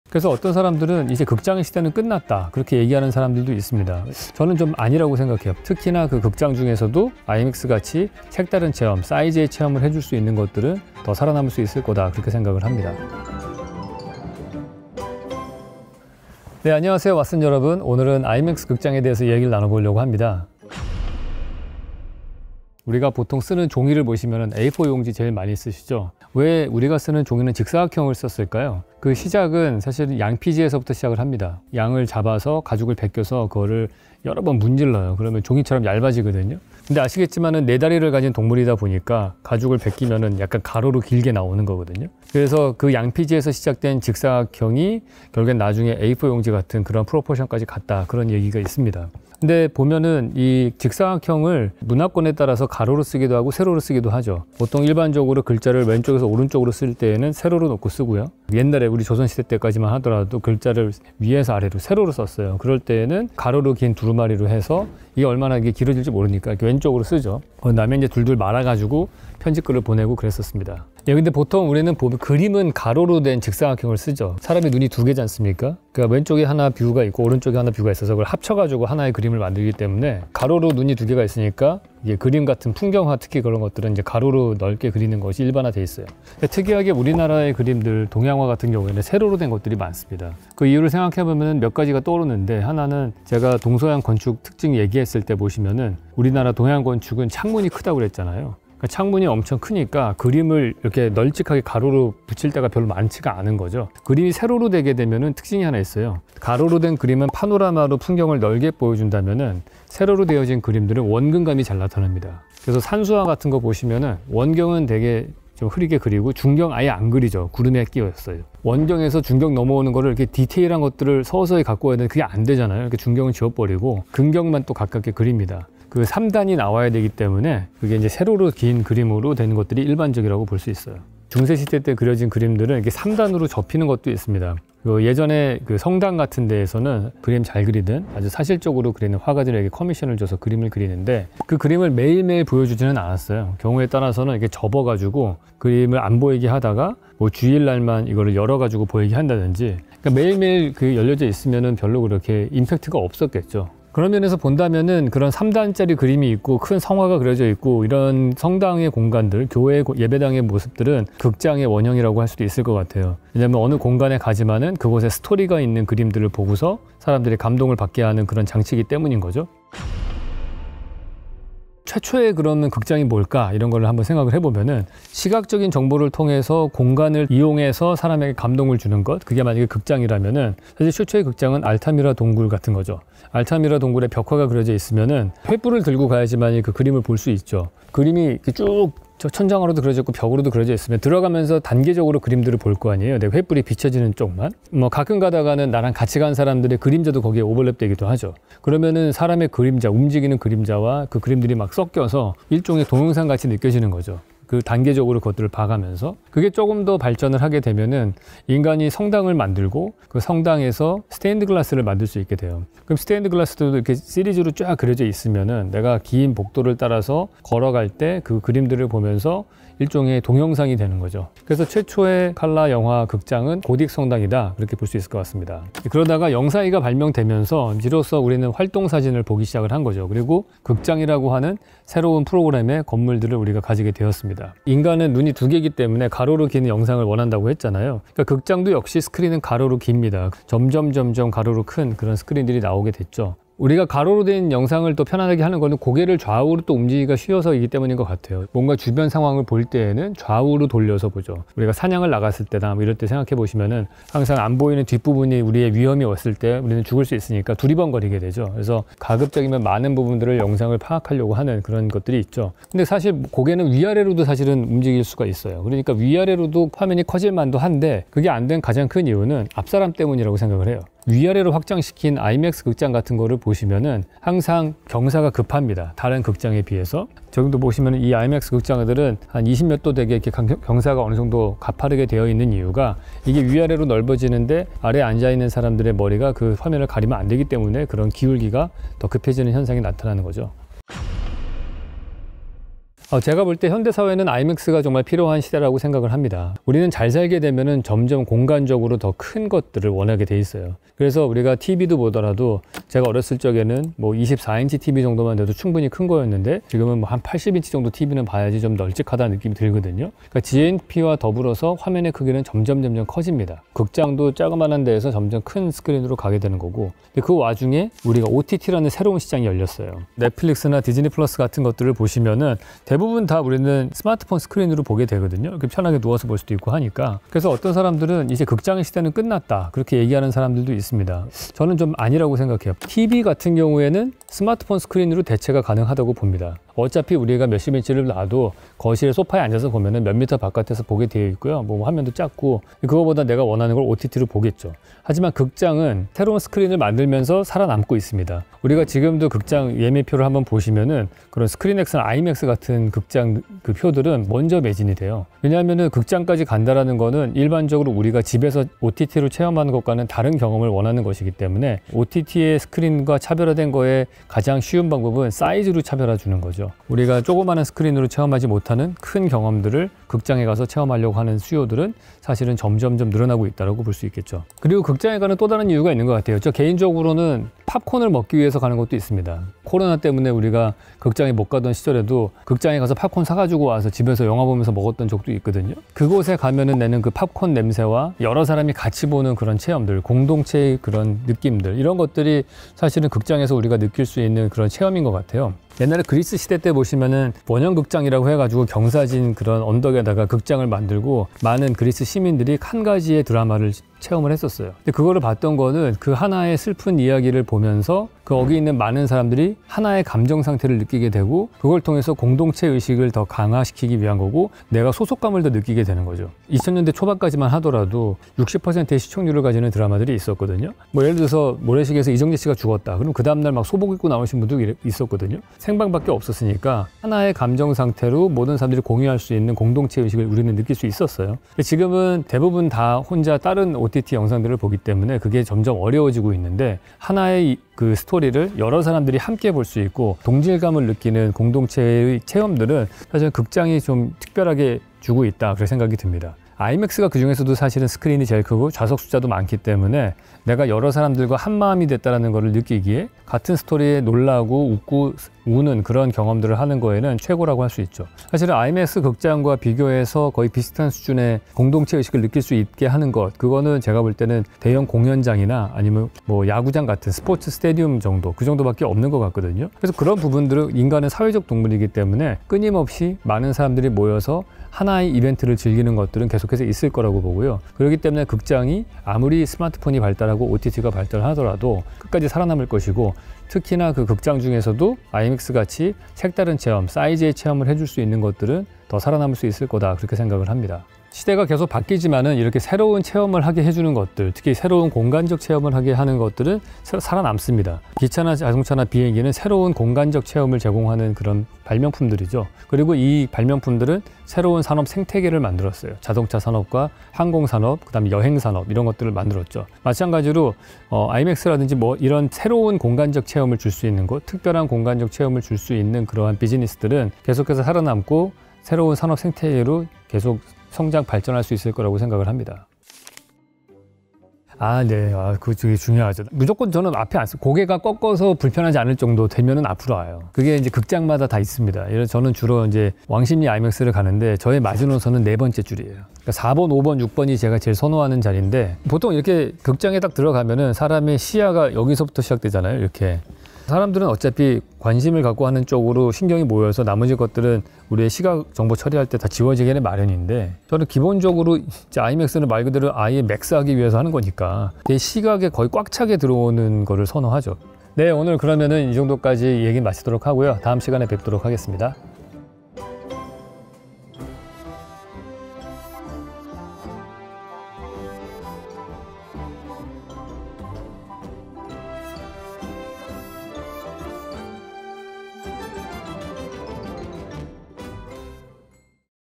[EN] 결국 영화관은 살아남는다.
왜 아이맥스(IMAX)만 항상 매진일까?
아이맥스의 차이 분석과 자리 추천까지!